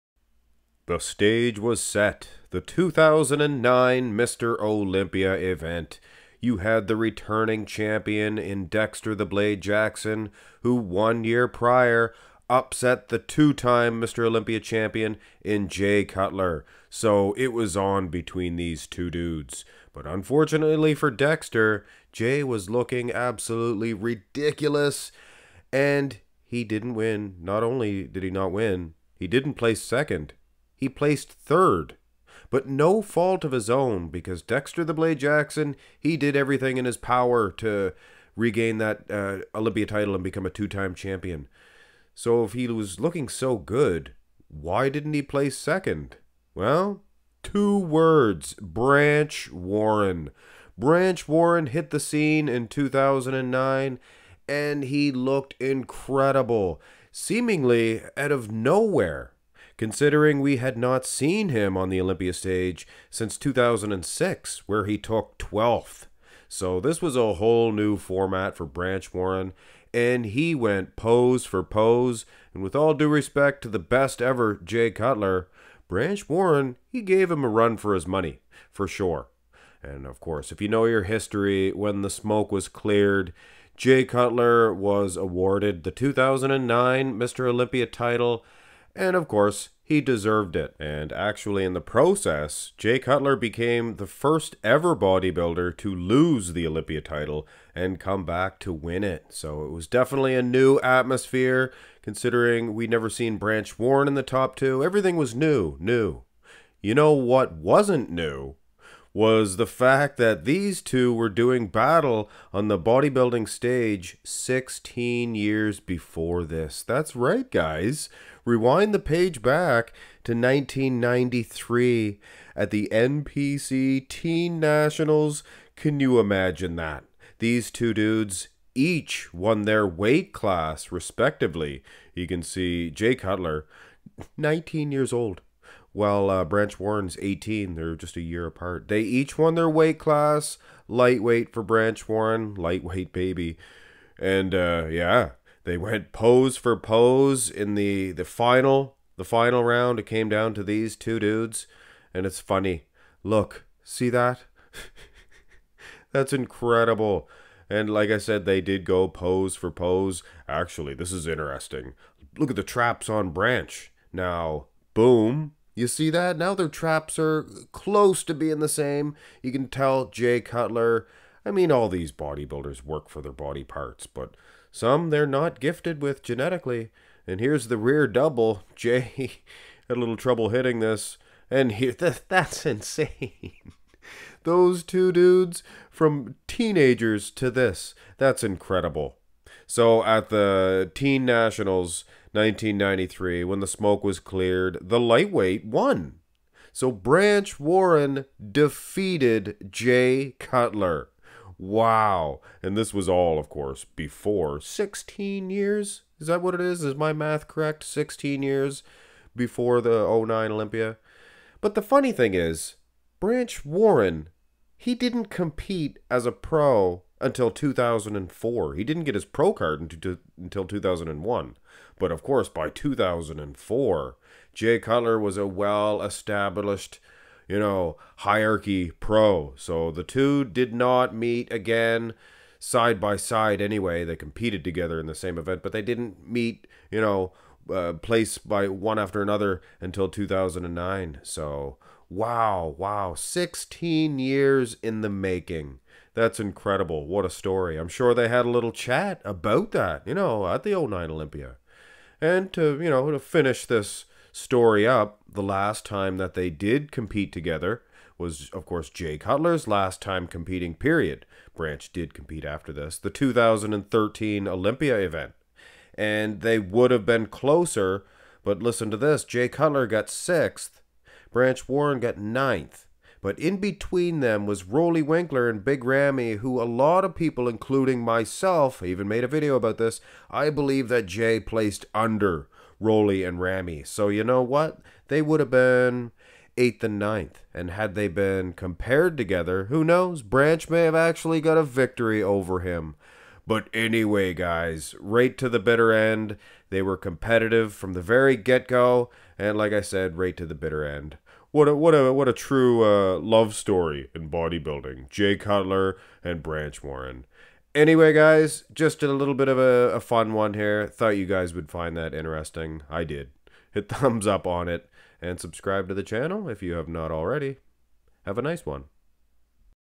Jay Cutler and Branch Warren *16 Years In The Making*
Right from the beginning of Jay Cutler's Bodybuilding Career until the very end, there has been a man he had to compete against in Branch Warren. This may be a shock to many of you that these two guys have been competing on stage beginning way back in 1993 if you can imagine that.